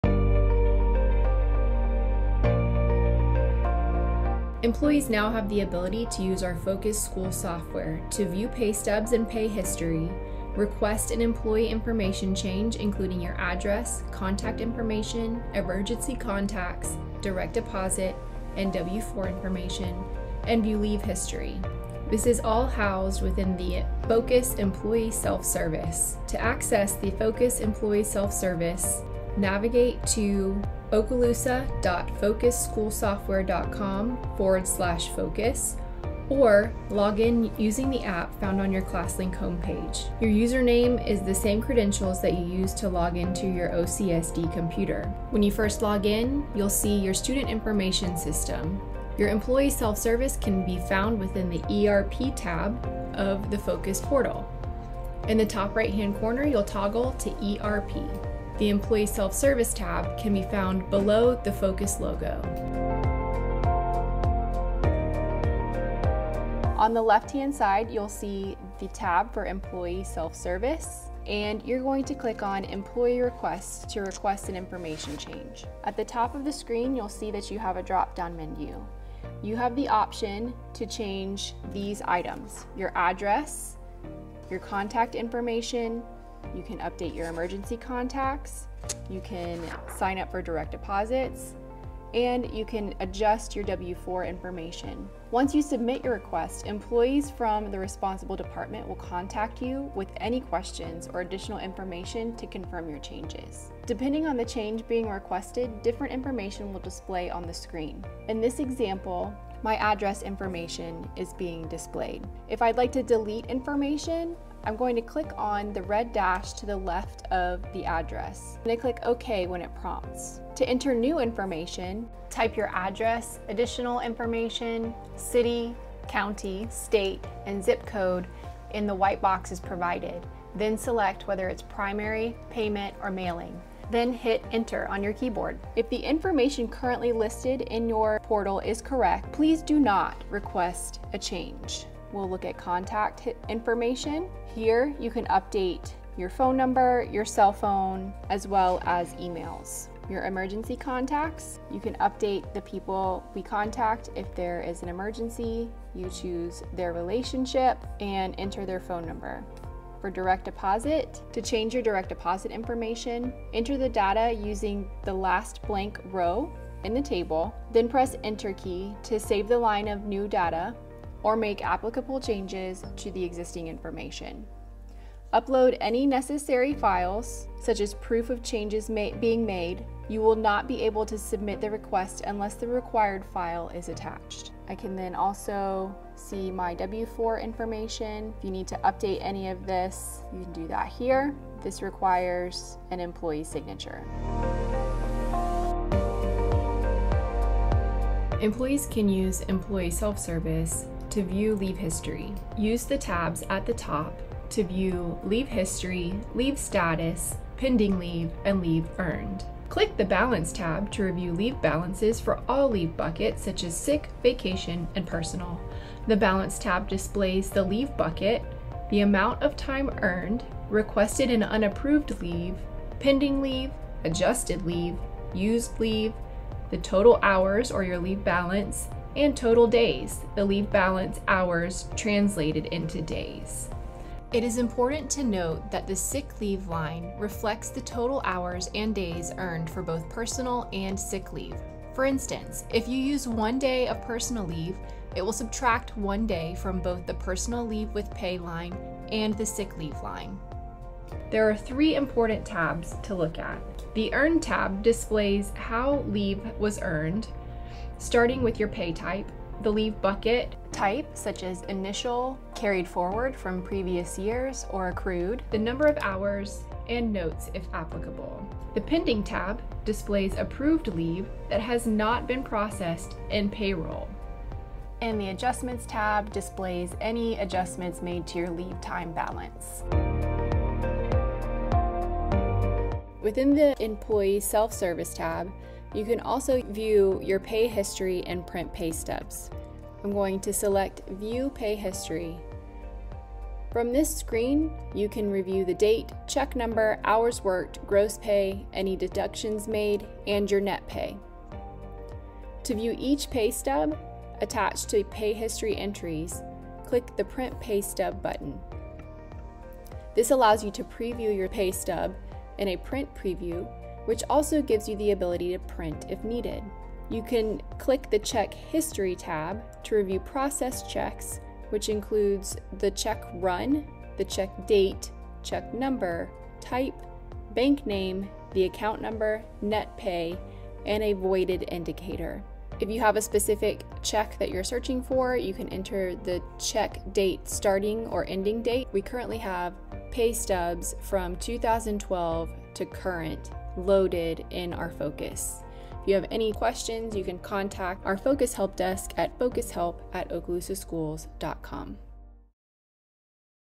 FOCUS ERP Employee Self Service
00:00 - Intro
01:03 - Employee Requests
03:15 - View Leave History
04:40 - View Pay History